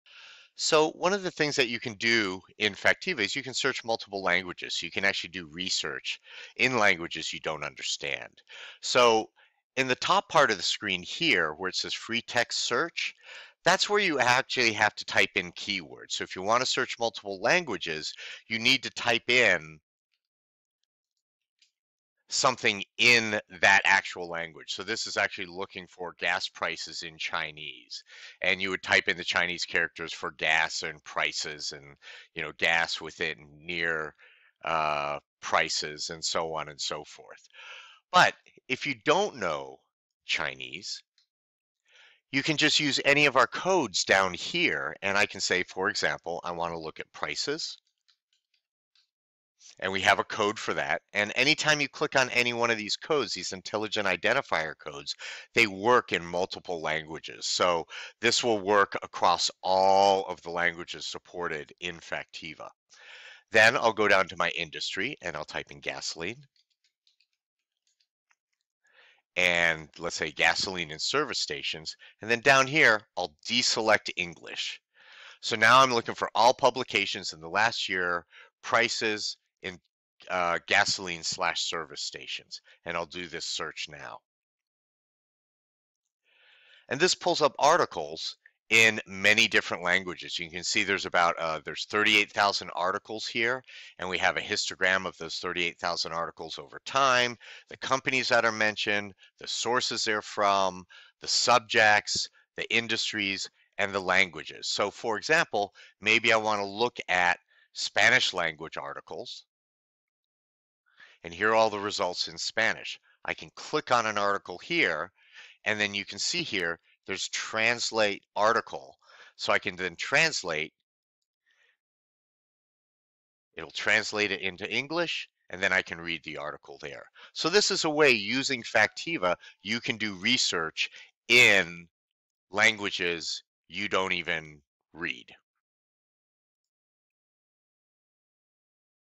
Factiva search in different languages
In Factiva the interface handles multiple languages in the free text search box, but if you don't know that language and spelling for keywords you can use our Intelligent Identifiers to create a search that works across multiple languages.